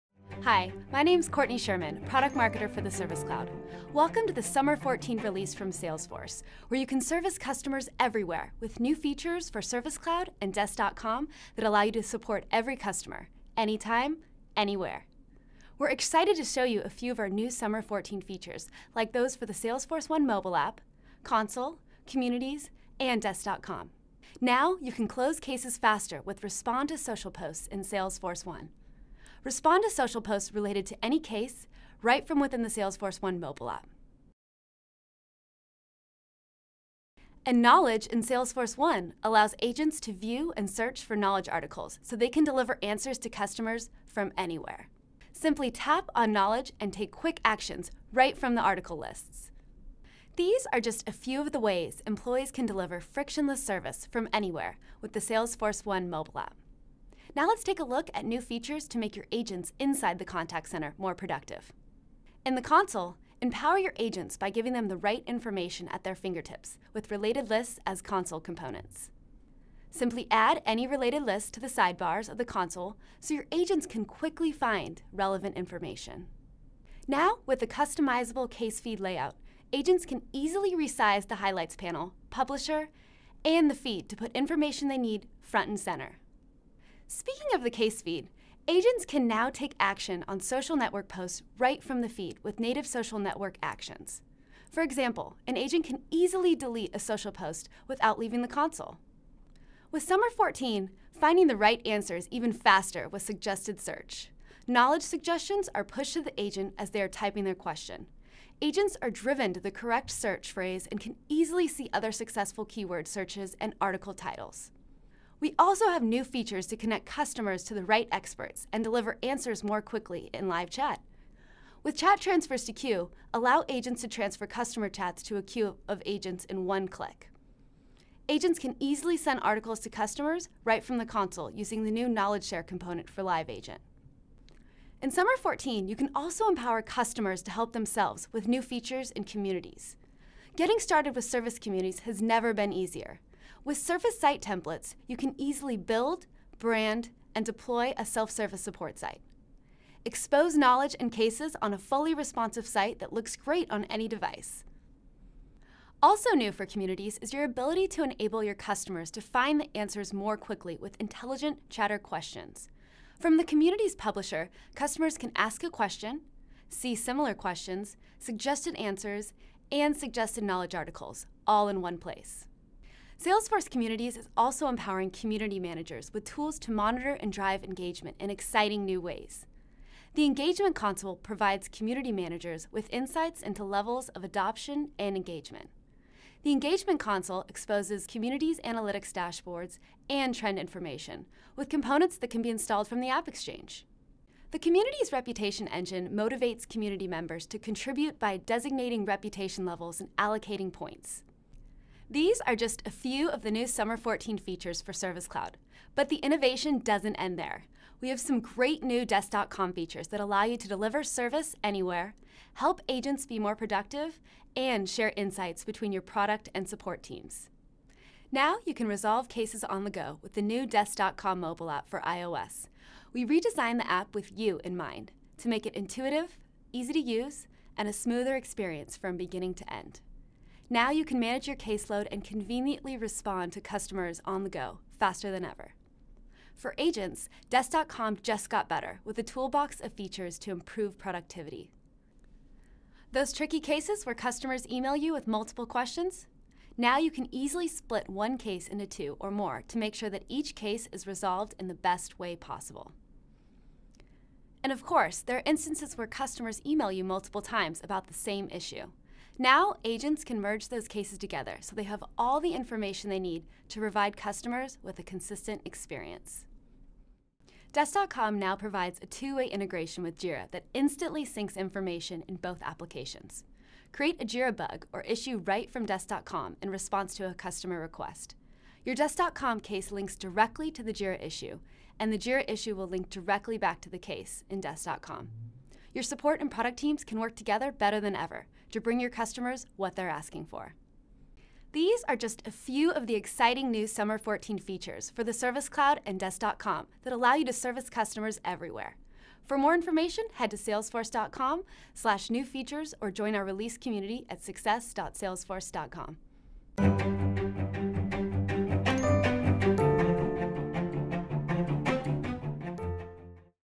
Service Cloud: Summer '14 New Features
Service customers everywhere with new Summer '14 features for the Service Cloud and Desk.com